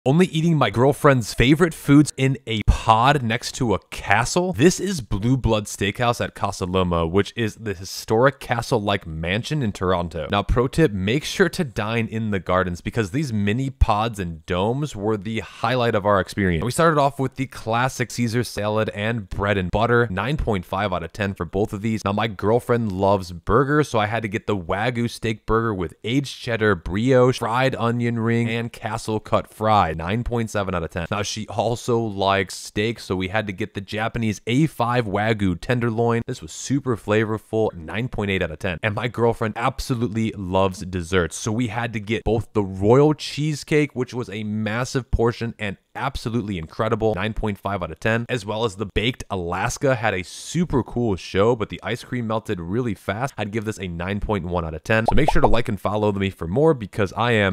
Only Eating My Girlfriend’s Favorite Foods for Dinner in a Castle?!
Only Eating my girlfriend’s favorite foods in a POD next to a castle?!

This is Blueblood Steakhouse at Casa Loma, which is the historic castle-like mansion in Toronto.

Now pro tip, make sure to dine in the gardens because these mini pods or domes were the highlight of our experience.

We started off with the classic Caesar salad and bread and butter – 9.5/10 for both of these.

Now my girlfriend loves burgers, so I had to get the Wagyu steak burger with aged cheddar brioche, fried onion, ring and castle cut fries. 9.7/10.

She also likes steak, so we had to get the Japanese A5 Wagyu tenderloin. This was super flavorful. 9.8/10.

And my girlfriend absolutely loves dessert, so we had to get both the Royal cheesecake, which was a MASSIVE portion and absolutely incredible. 9.5/10.

As well as the baked Alaska had a super cool show, but the ice cream melted really fast. I’d give this a 9.1/10.

Make sure to like and follow for more because I am…

Would you try these foods at the Blueblood Steakhouse?